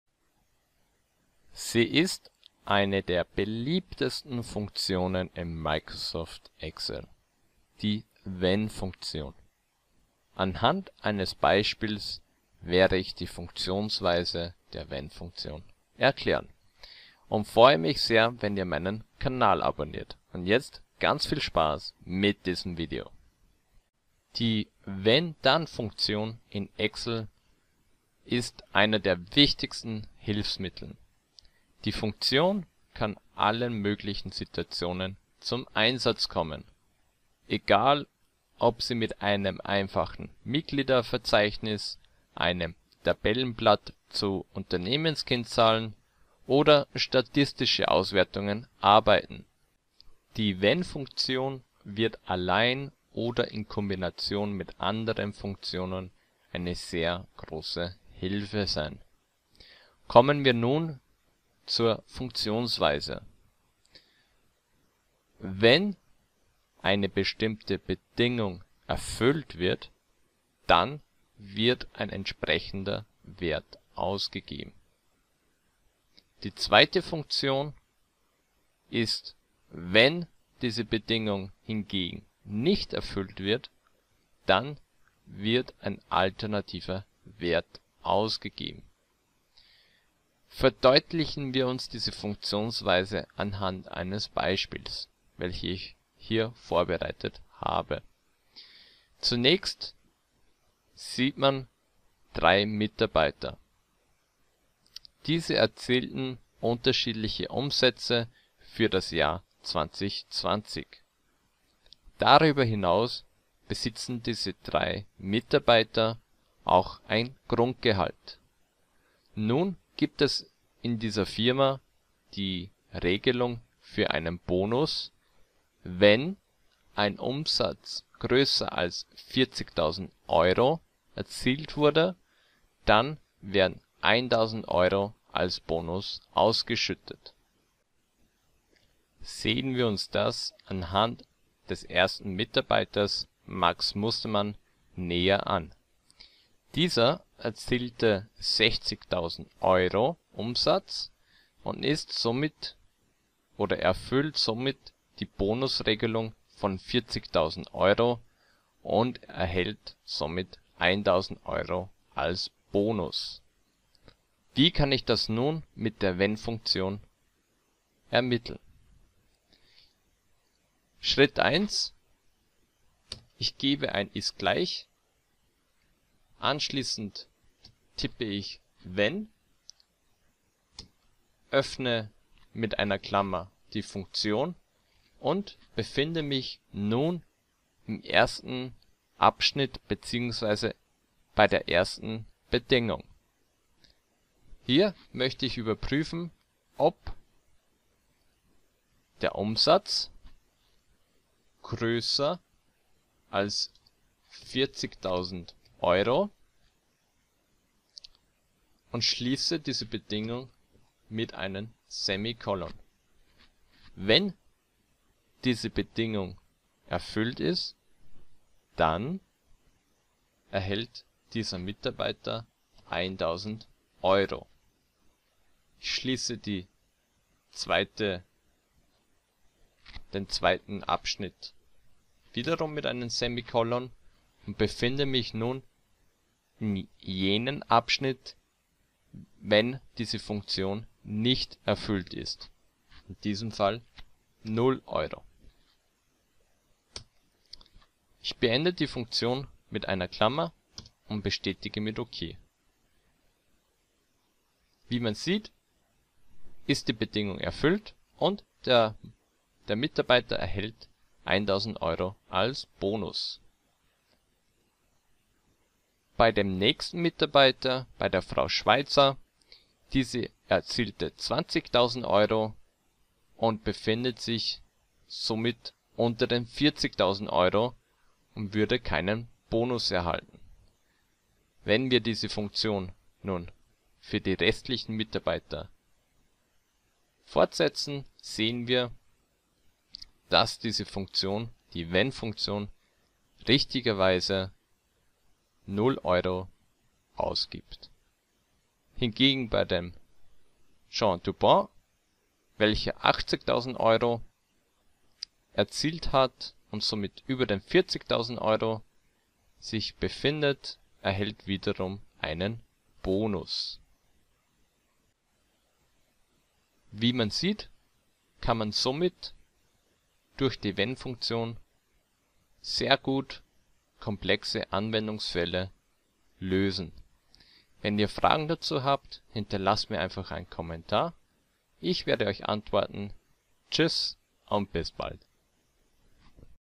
Excel Wenn (IF) Beispiel
Sie ist eine der beliebtesten Funktionen in Excel: WENN. Damit gehört Wenn-Dann in Excel zu den wichtigsten Hilfsmitteln. Die Funktion kann in allen möglichen Situationen zum Einsatz kommen